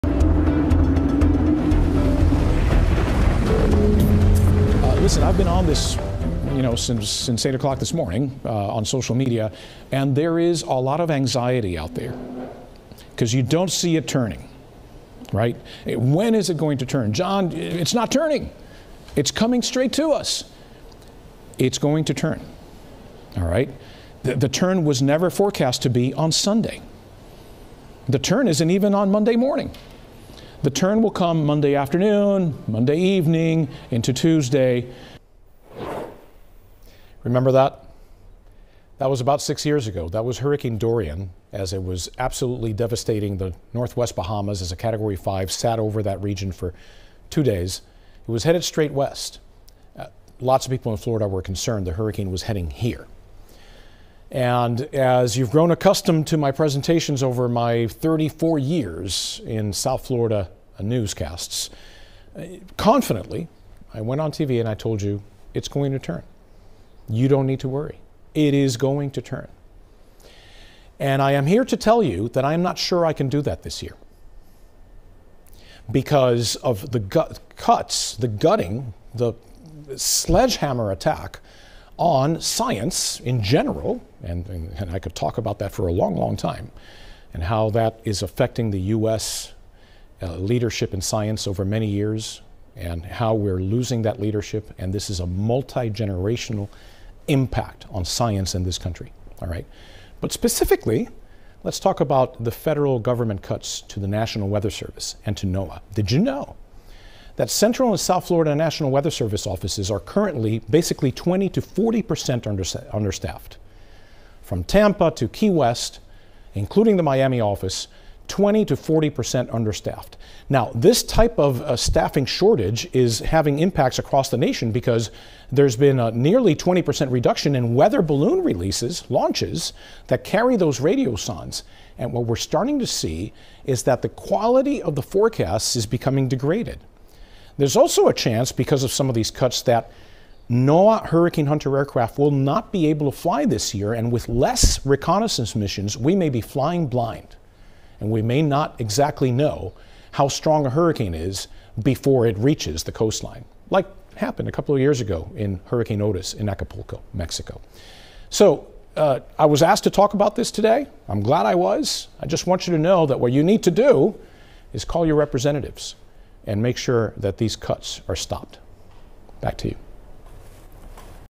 John Morales on the impact of government cuts to the National Weather Service
John Morales breaks down how recent cuts to the National Weather Service and the National Hurricane Center at NOAA have left them understaffed and degraded their ability to forecast hurricane strength and trajectory. MUST WATCH!